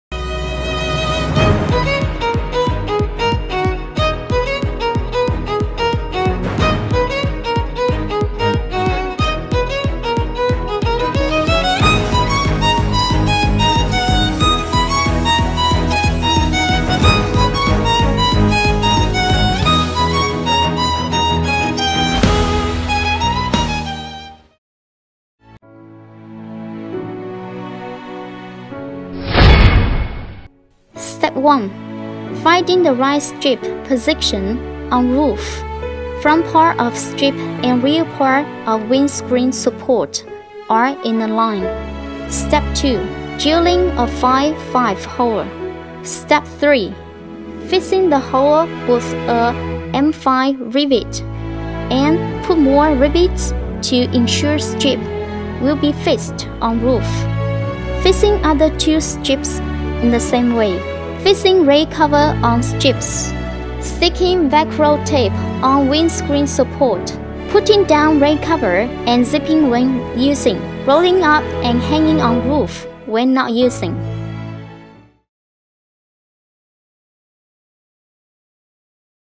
LVTONG Golf Car Assembling rain coverstrip type Taking A8 for example
This video gives instruction about how to assemble rarin cover. If you want to know more about lvtong golf cars, sightseeing buses, etc.